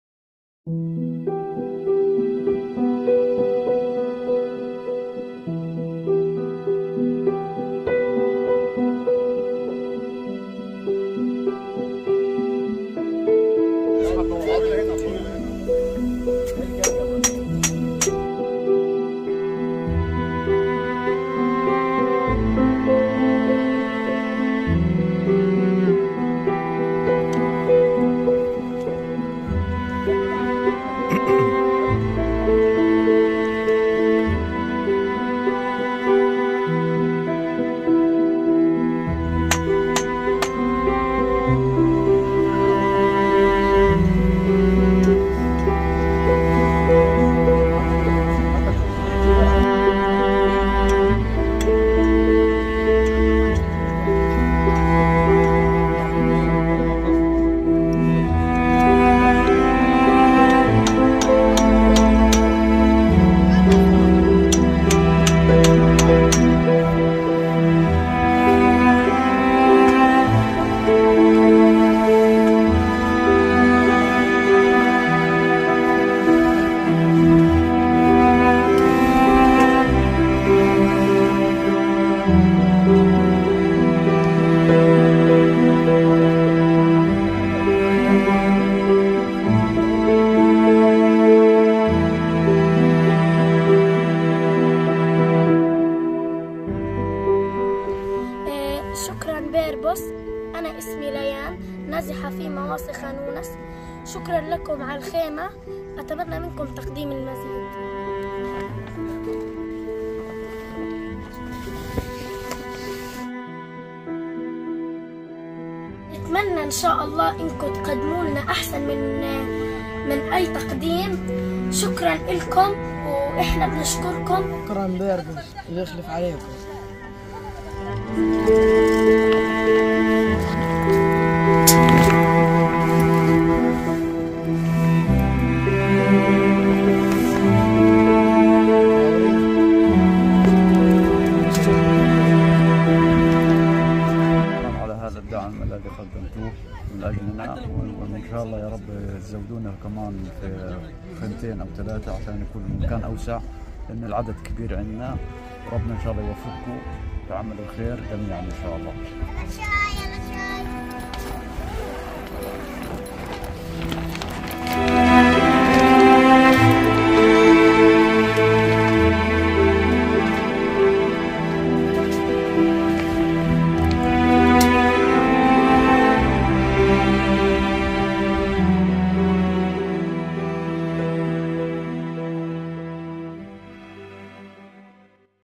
GAZA - TENTS! - BATCH 157
The struggle continues for the Palestinians and so does the aid from Purpose of Life!

Please keep supporting this blessed cause.